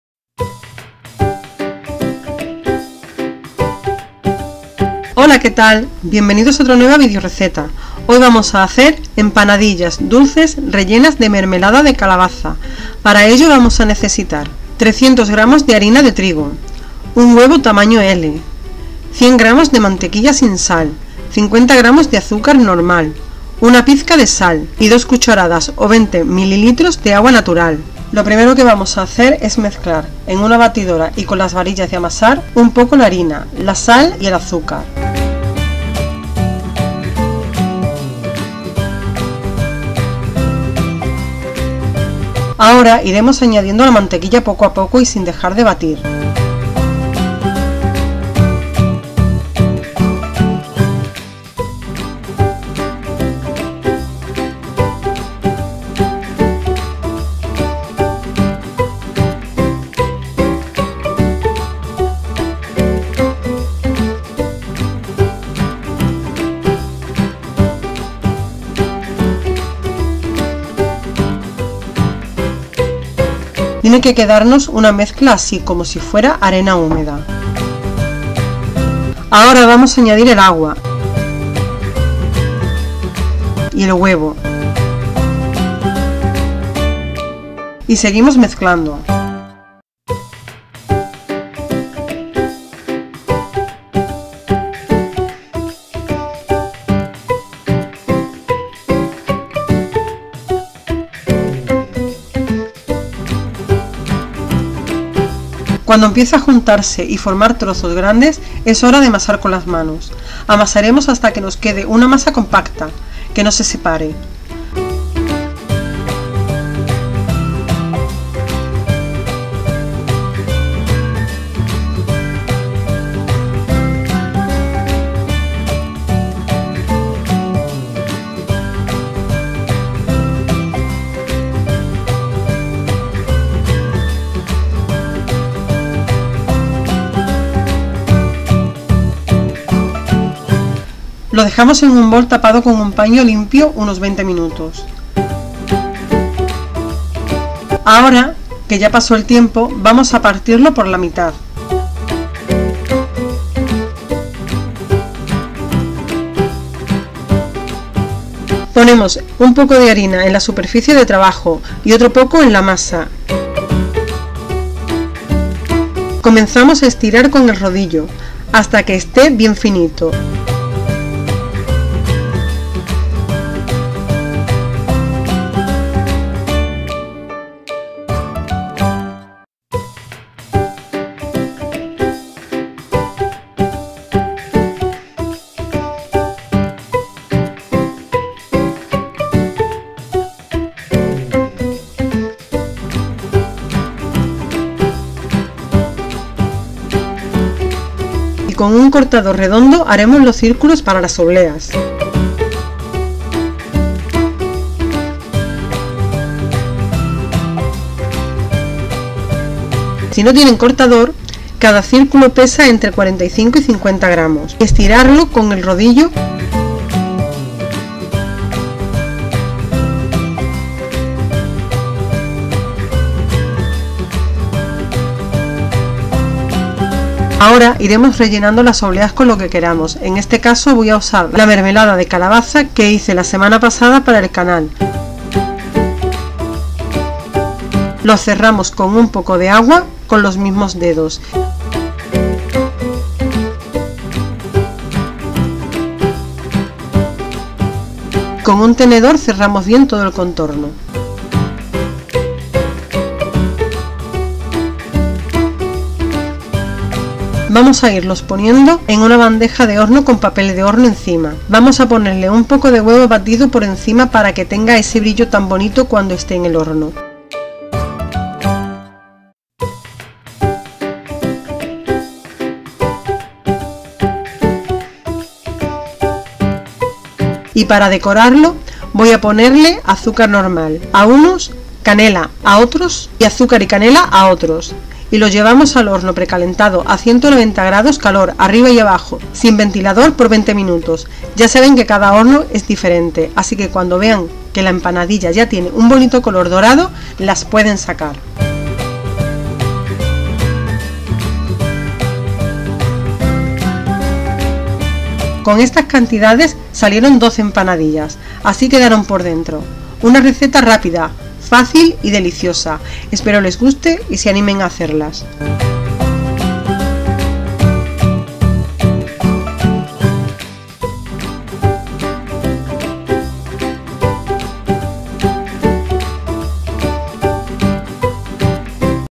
Empanadillas Dulces Rellenas de Mermelada de Calabaza
Cocina Dulce y Salada By Shary

Empanadillas Dulces Rellenas
de Mermelada de Calabaza
Ingredientes para 12 Unidades:
300grs. Harina de Trigo Normal
1 Huevo Tamaño L
100grs. Mantequilla Sin Sal
50grs. Azúcar Normal
Una Pizca de Sal
2cdas. o 20ml. Agua Natural

NOTAS:
Si la mantequilla que usan es con sal, omitan la pizca de sal.
Las obleas pesan 45/50grs. así si no tienen cortador redondo,
pueden pesarlas y estirarlas con el rodillo intentado dejarlas
lo más redonditas posibles.

Cualquier pregunta, duda, dejamela en los comentarios.